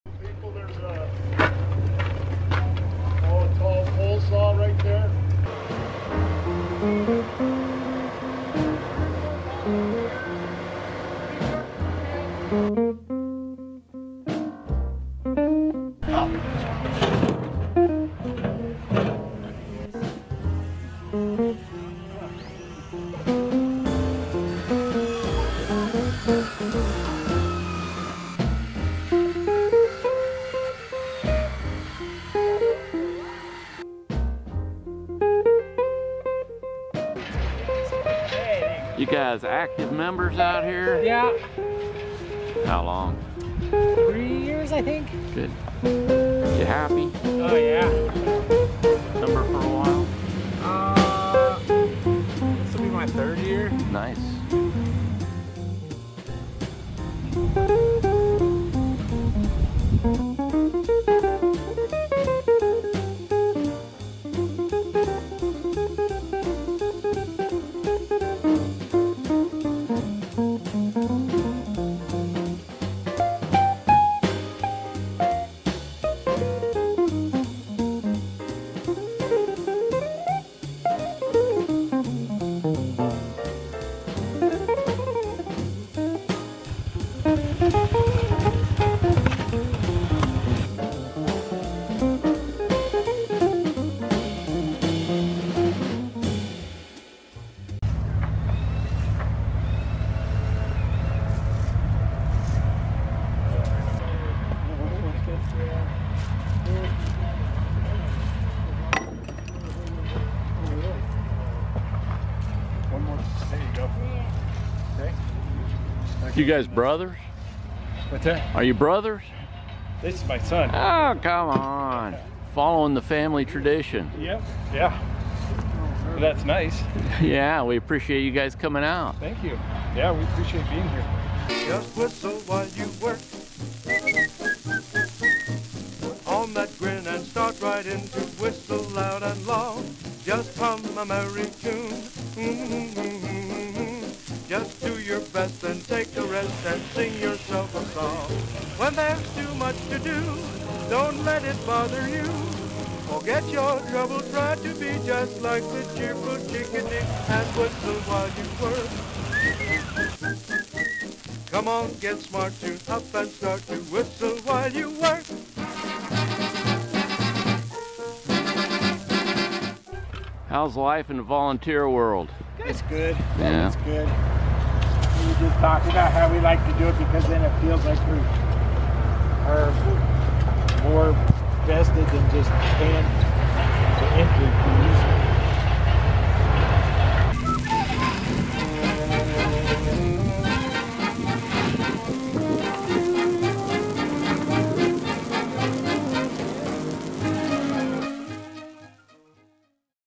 Golden High Country Archers 2023 spring workday
We had a record breaking ninety-five volunteers show up to help us out this spring! Here's a sample of what we got done.